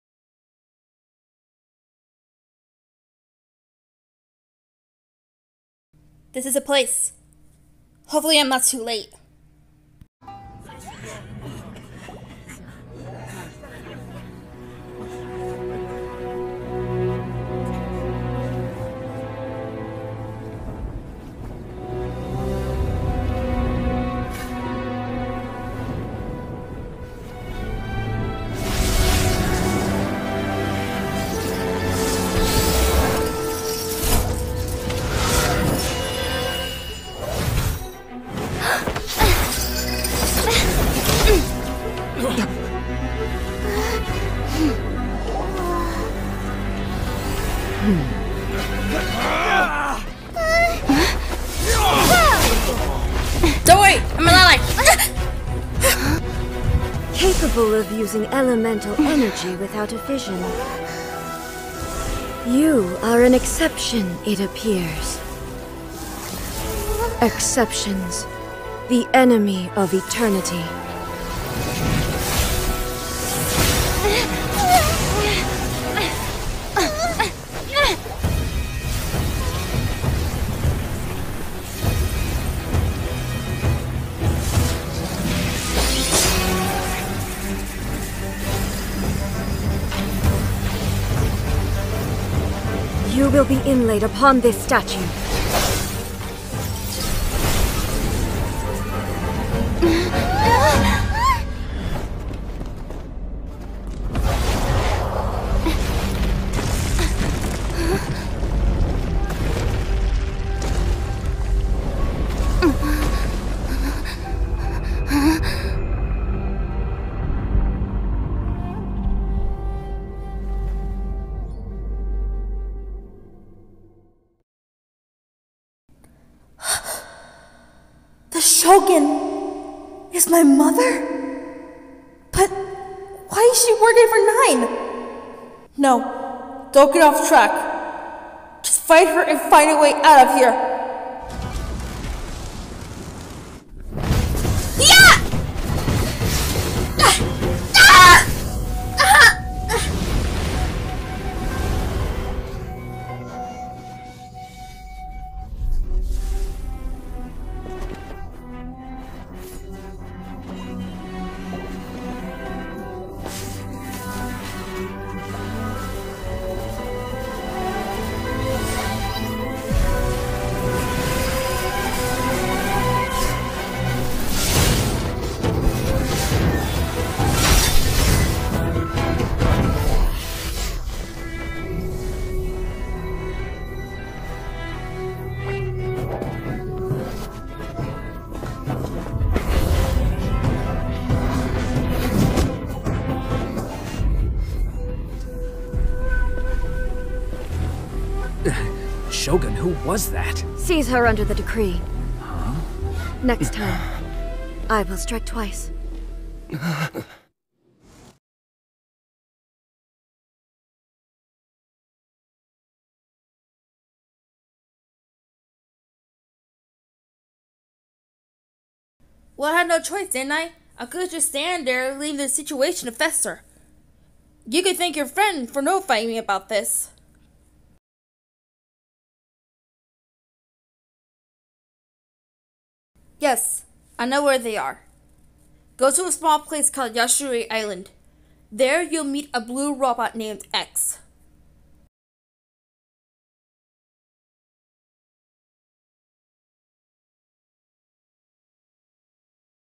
Heroes Day Aftermath Part 3- Rescuing Erica
(This Part does contain spoilers from the Inazuma Archon Quest. So be cautious when watching this. Also I worked on this part before part 2, so the plotline is gonna be a bit rocky)

Here's part 3. Hope you like it!

Story: Bella goes a statue where a ceremony is taken place. Margot's friend, Erica who's arms are tied behind her back looks up to the one hosting the ceremony. Bella leaps into action by grabbing the vision and look up to the host. She runs to Erica, about to untie her. But was stop, by the Shogun telling her that she was the Enemy of eternity. She pulls a sword out and sends Bella to another dimension, where she saw her mother. At the end of the battle, she was struck down. Before death was flashing before her eyes, Erica gets herself untied from the spear that the guard dropped and throw the spear. Bella's mother then slashes the spear and Erica catches Bella. She looks at Bella and Erica for the last time and they both flee. Her orders? We're to seize Bella under the decree.

Characters by appearance

My Mom (portrayed as Raiden Shogun and Raven branwen)
Bella
Erica (Portrayed as Tari)